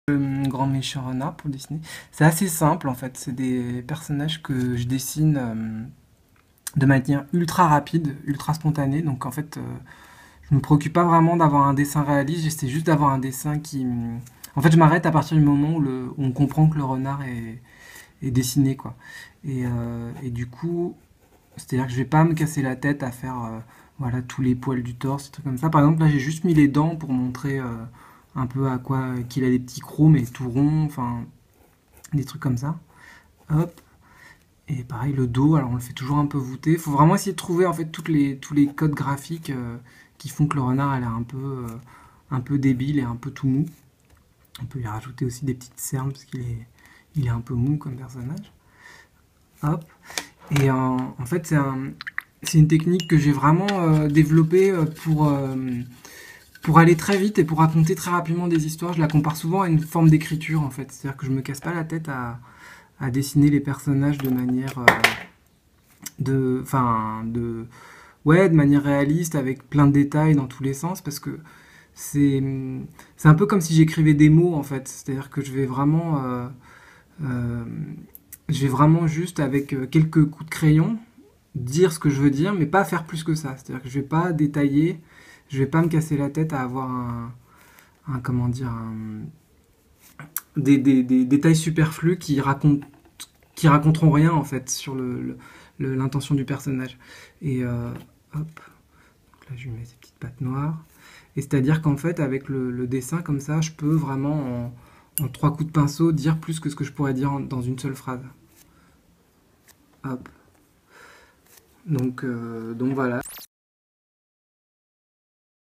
Benjamin Renner : comment dessiner "Le grand méchant renard" ?
Benjamin Renner adapte à l'écran en dessin animé sa bande dessinée "Le Grand méchant renard" paru chez Delcourt. Il explique ici à Anne Douhaire comment il a dessiné son personnage.

Le Grand méchant renard, un film produit par FOLIVARI /PANIQUE!/STUDIOCANAL /RTBF(Télévision belge) - OUFtivi /VOO /Be tv